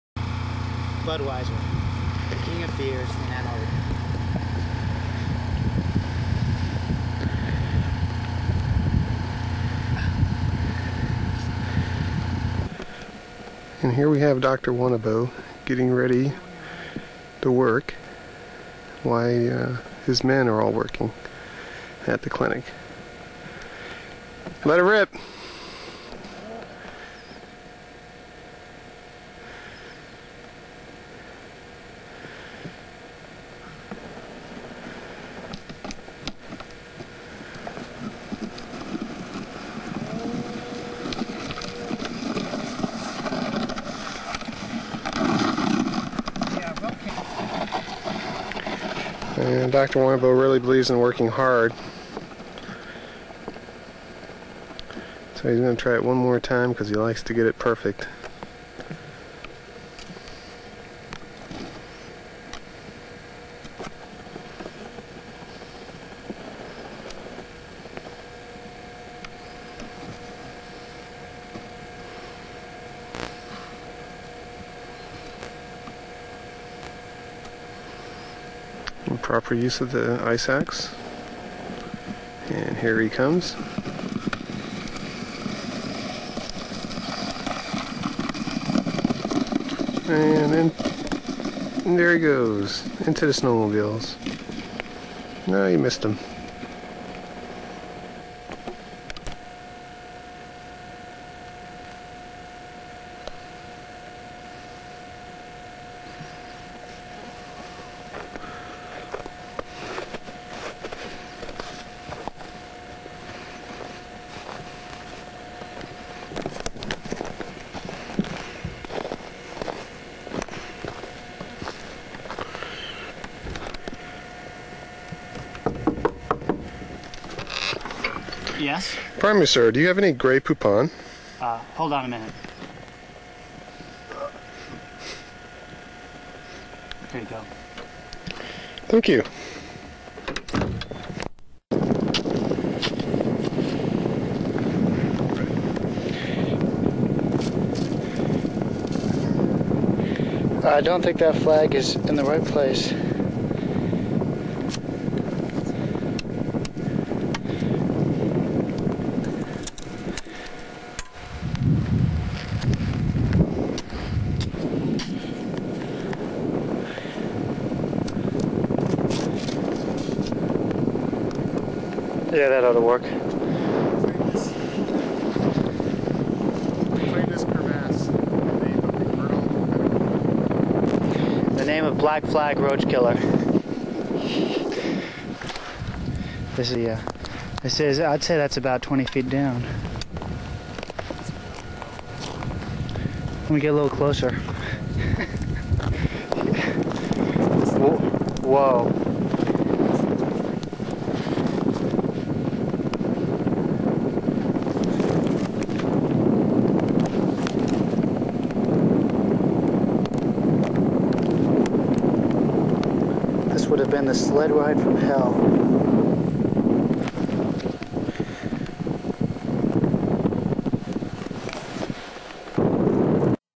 Operation Deep Freeze 92 camp on Ross Ice Shelf Crevasse marking
we spent a lot of time camping in scott tent on the ross ice shelf and see me doing some crevasse marking for the camp and some fun video stuff out on the ice